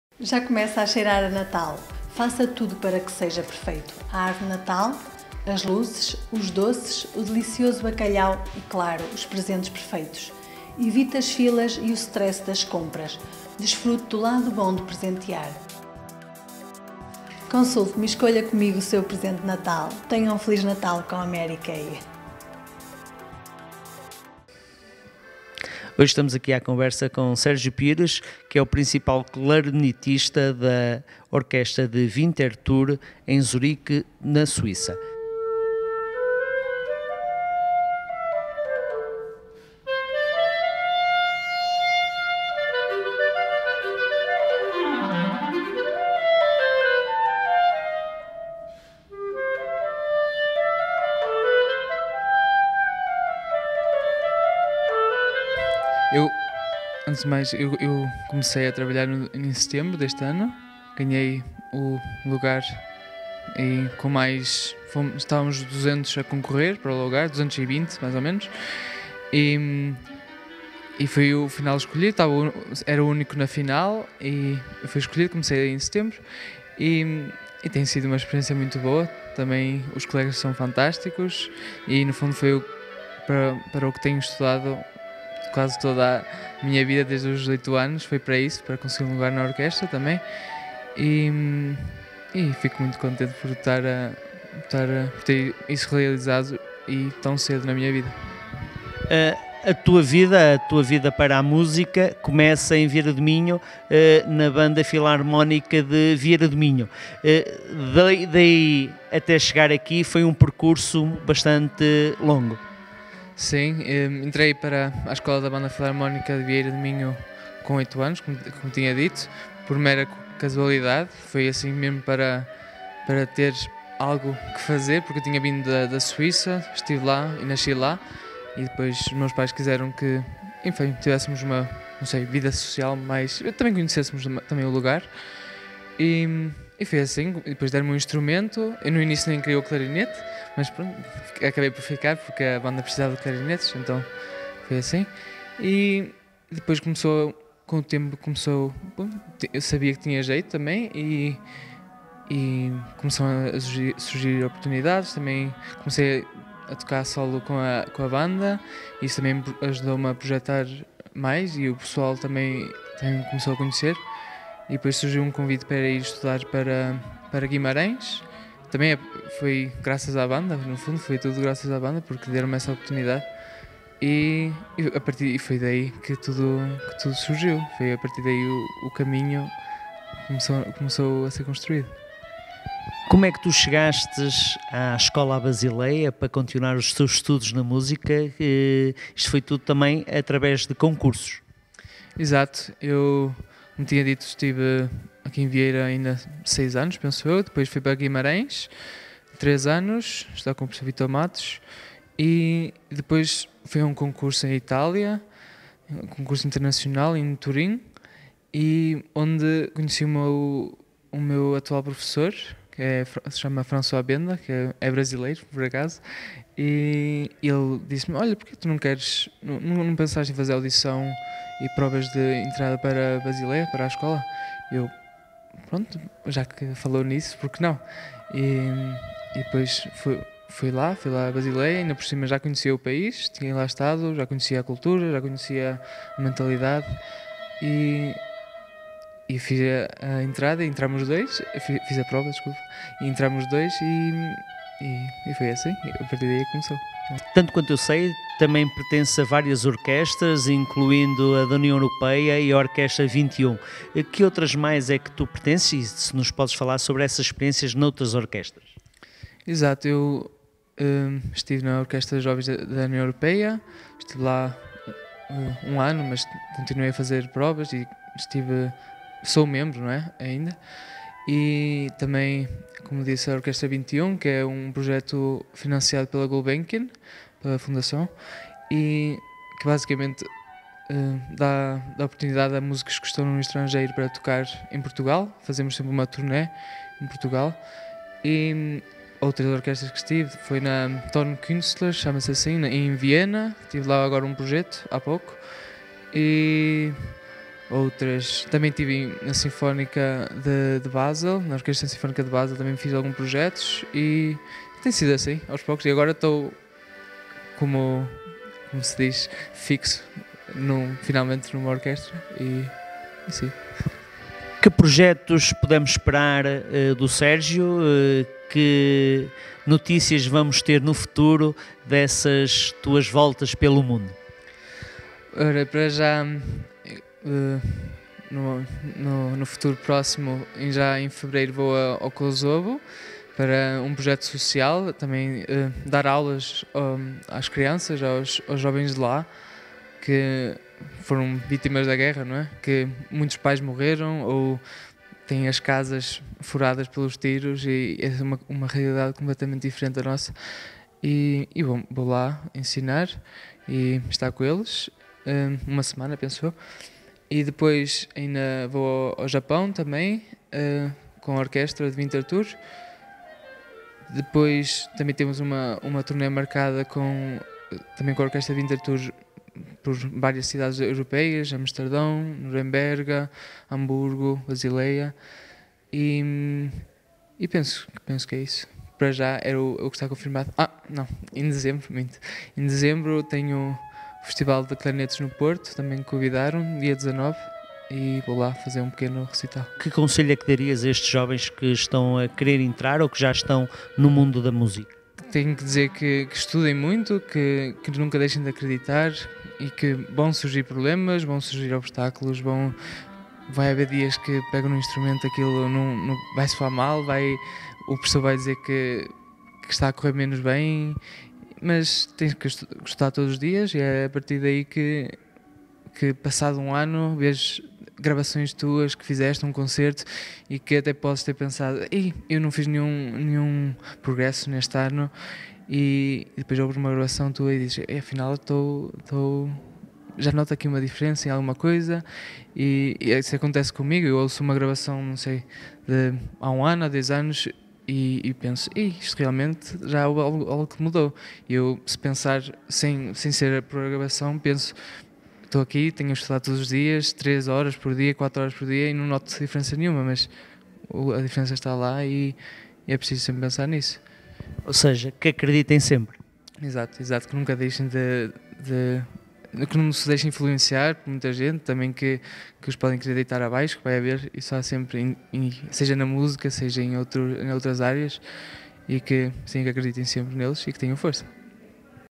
Sérgio Pires
Jovem músico Vieirense dá cartas na Orquestra Winterthur
A Orchester Musikkollegium Winterthur é uma conhecida orquestra suíça, fundada em 1875 com o nome de Stadtorchester Winterthur. Precedido pelo austríaco Heinrich Schiff, seu atual regente titular é o holandês Jac van Steen.

Descubra agora um pouco da vida do Sérgio.